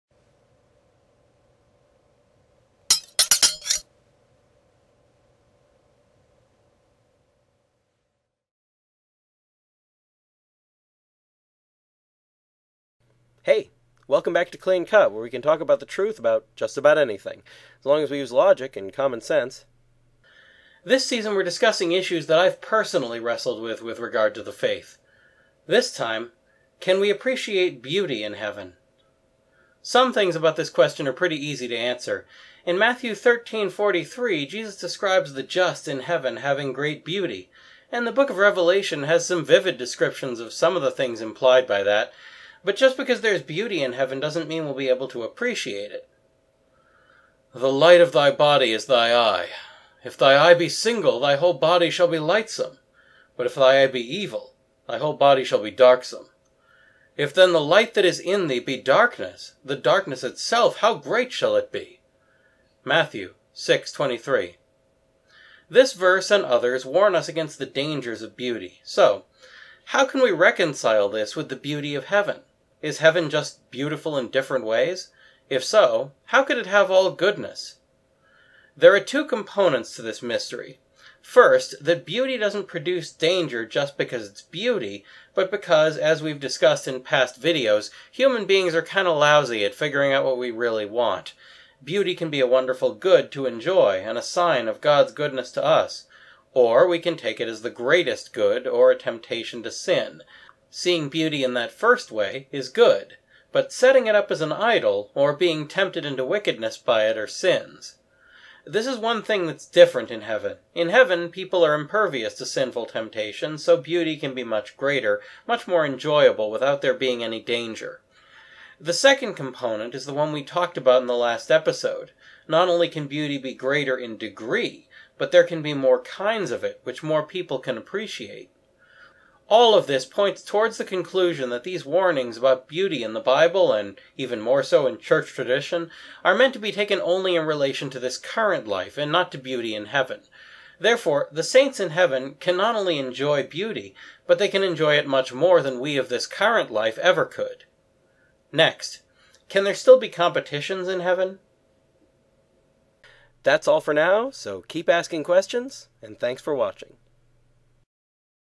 Can People in Heaven Have Beauty?
Does beauty lead to sin?  What's the problem with beauty?  Why is some beauty dangerous?